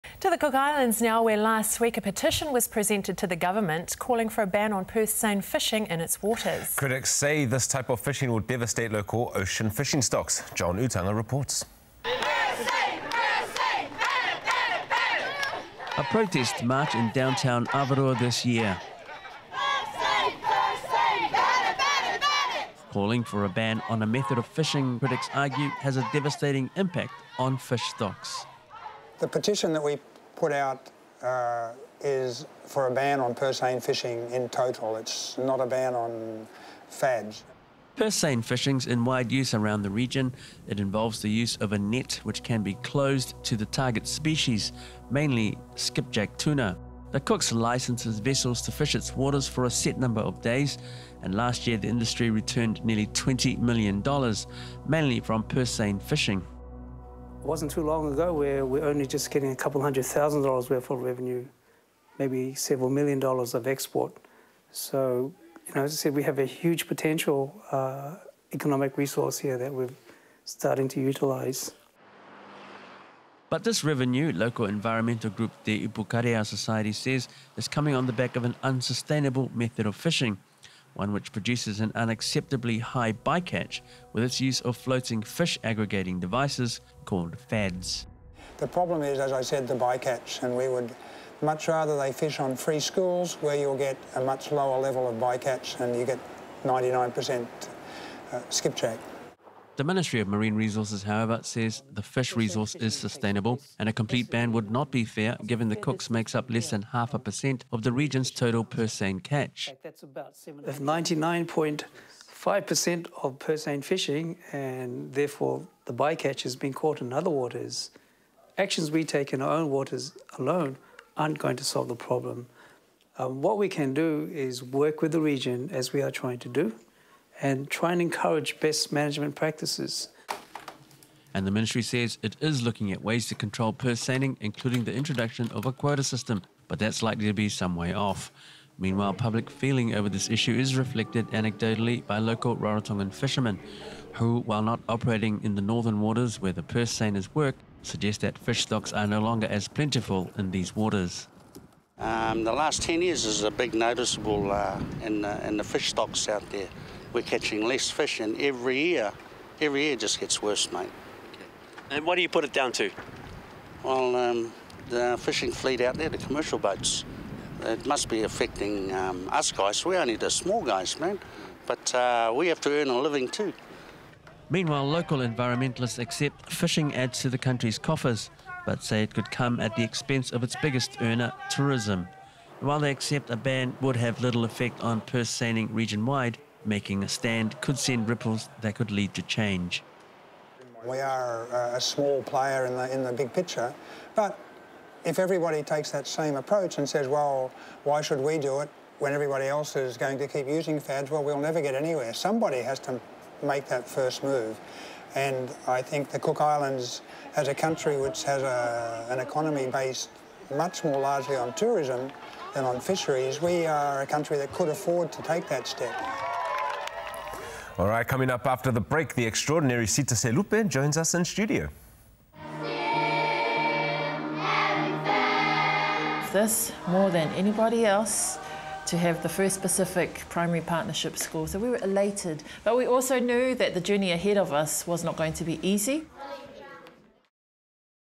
Cook Islands Petition calls for ban on purse seine fishing ...
Petition calls for a ban on purse seine fishing but the Cook Islands government disagrees … John Utanga has more